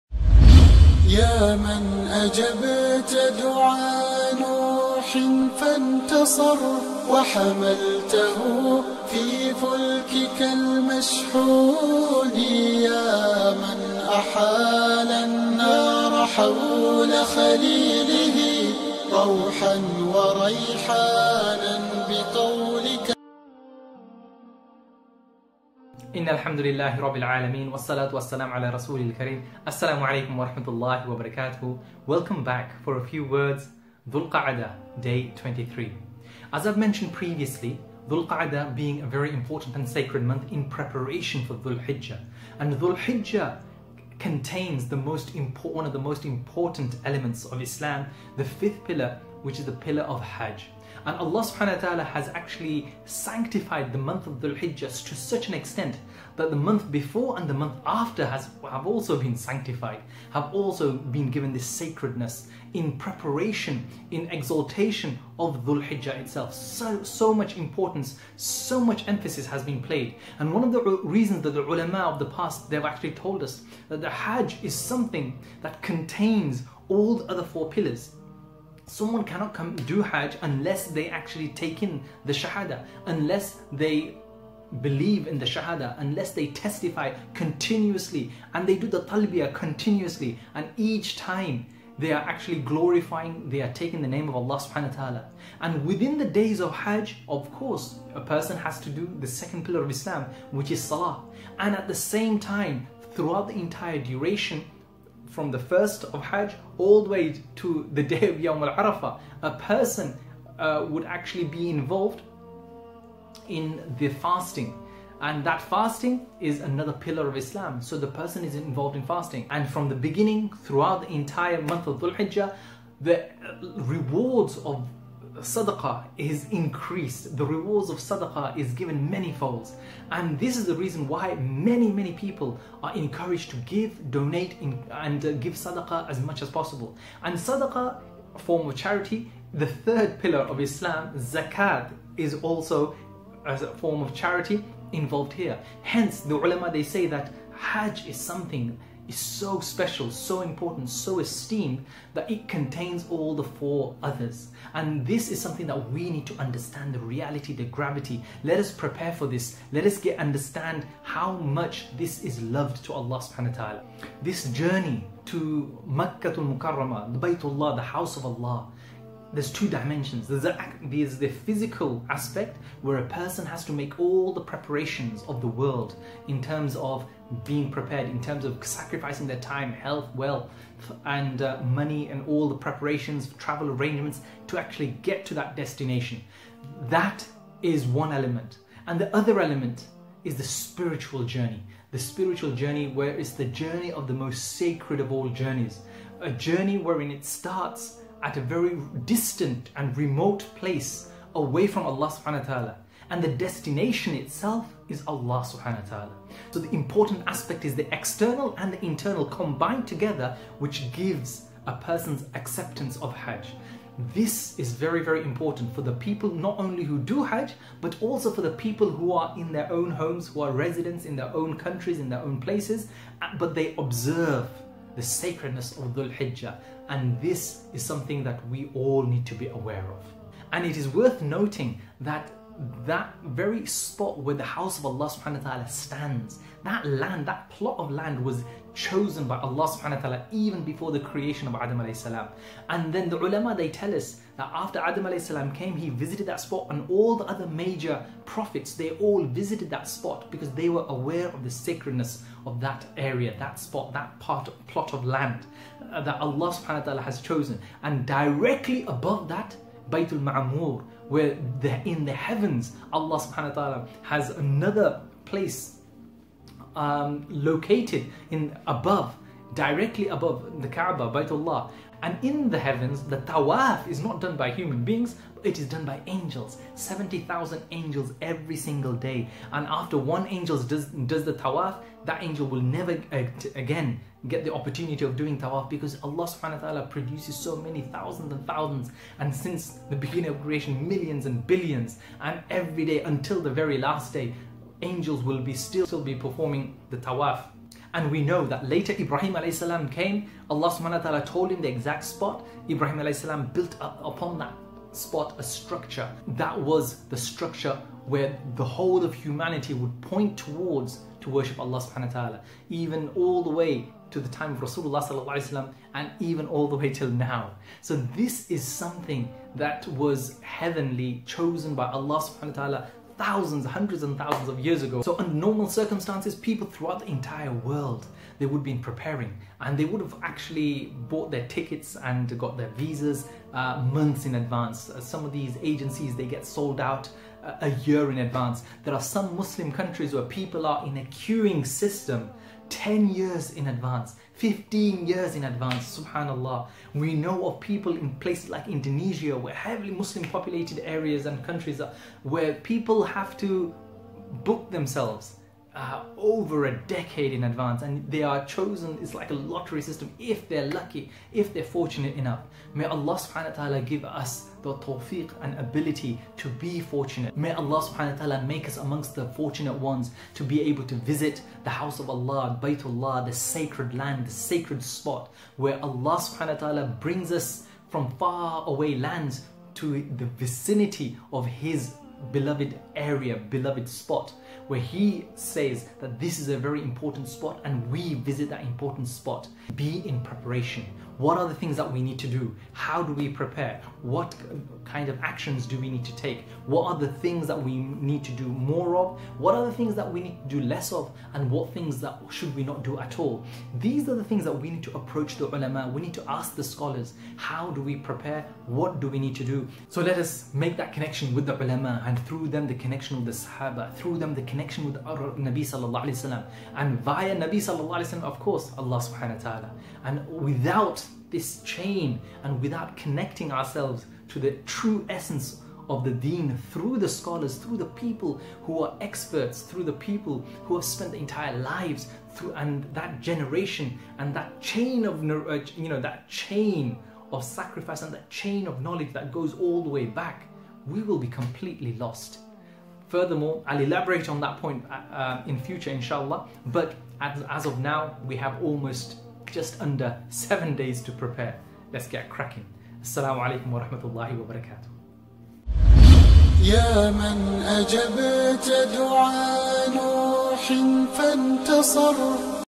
The Pillar That Contains All Pillars, The Journey To Allah ...A Few Words Dhul Qa'dah 23
The importance of  observing the ibaadat of Dhul Hijjah and the pivotal position of the Ka'bah. With less than a week away, preparation needs to start now. the significance of connecting with the ulamah to clarify these actions.